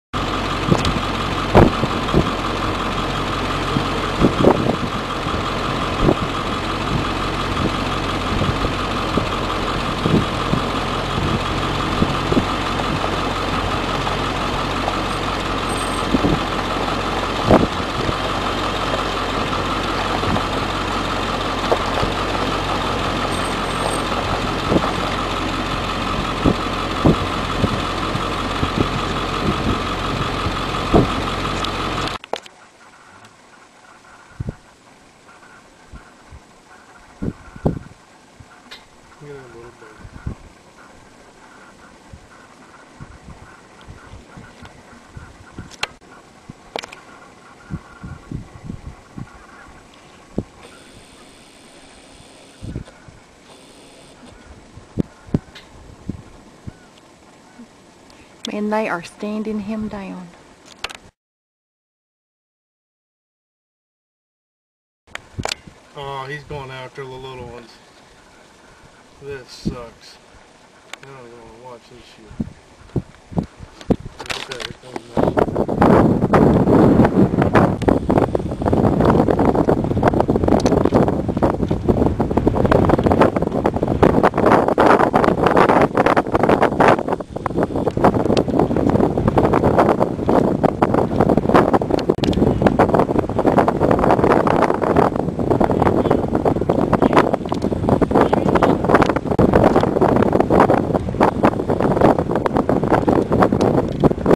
Bear and Musk Ox
A resident grizzly bags a couple of musk ox calves in Prudhoe Bay, Alaska. The day after this video this same grizzly took down an adult more than twice her size!